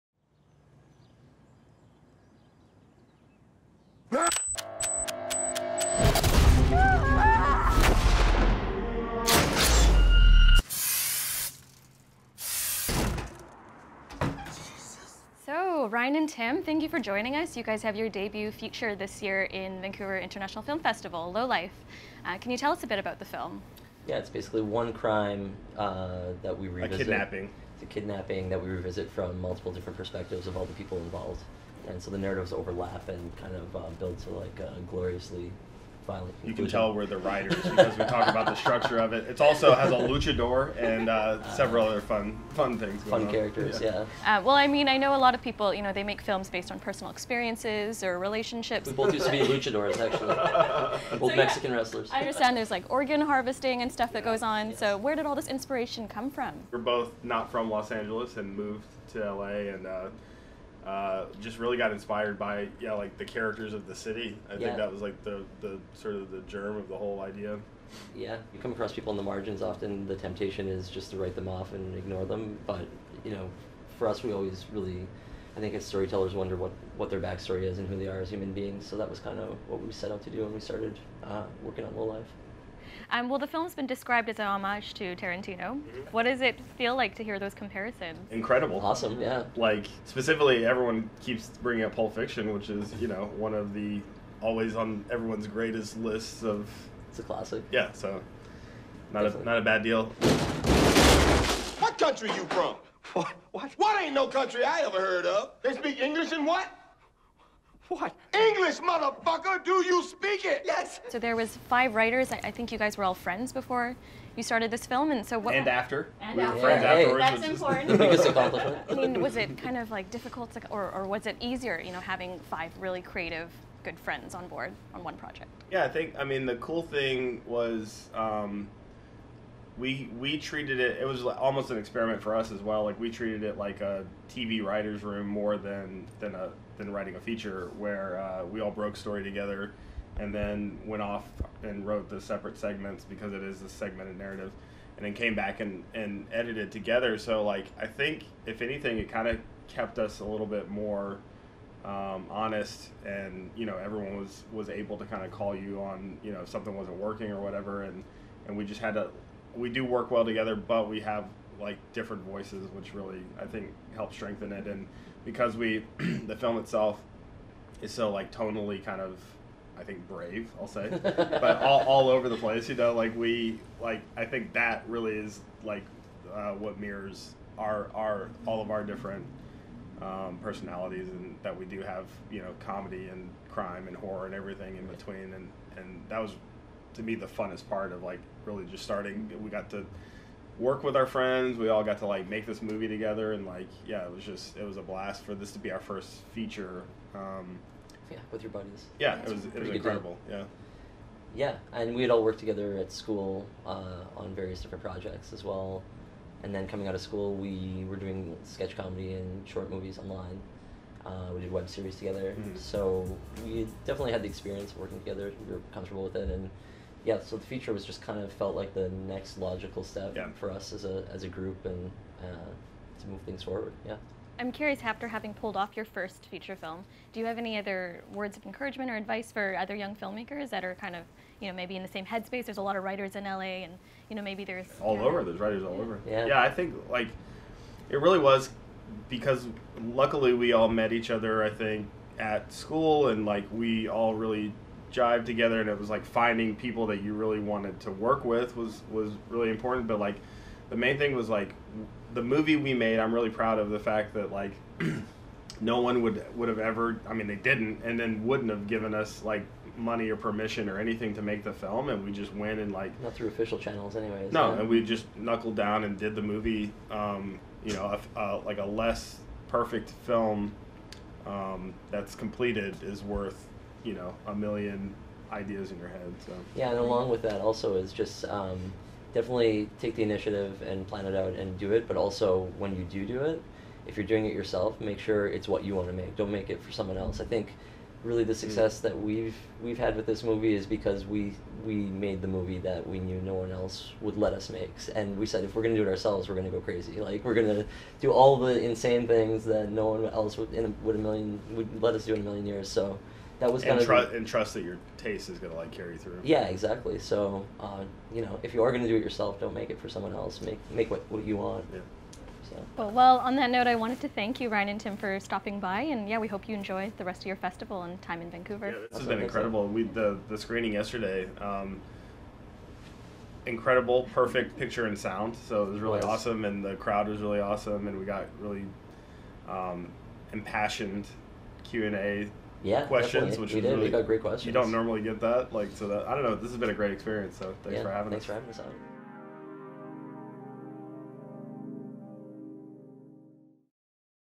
'Lowlife' - In Conversation with Ryan Prows & Tim Cairo | VIFF 2017
Ryan Prows, writer/director and Tim Cairo, writer/producer of 'Lowlife, a part of VIFF 2017's ALT stream.

Host: Devon Wong